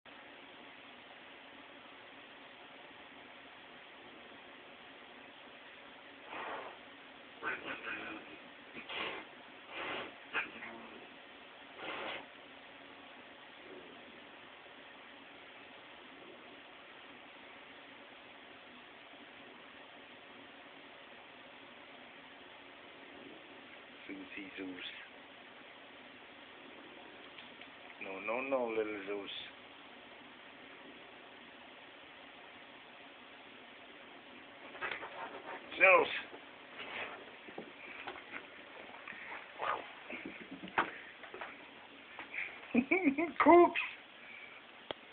zeus wants to play
zeus just wants to play but jeannie does not lol